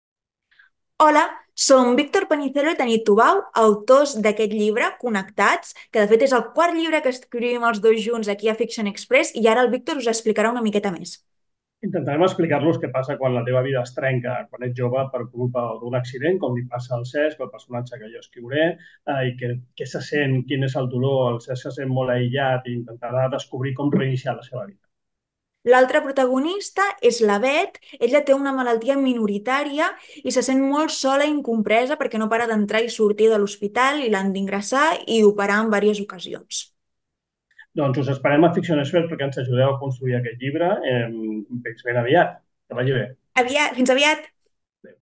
Víctor Panicello, Tanit Tubau - Connectats - Nivell 2
La Beth pateix una malaltia minoritària, l’arteritis de Takayasu, i ha de passar llargues estades ingressada en un hospital de Barcelona. El Cesc, a conseqüència d’un accident de cotxe, en què ha perdut els seus amics, està ingressat en un hospital de Ciutat de Mèxic, on resideix actualment. Tots dos se senten sols, però malgrat la distància connectaran i aconseguiran abandonar la seva illa de la solitud.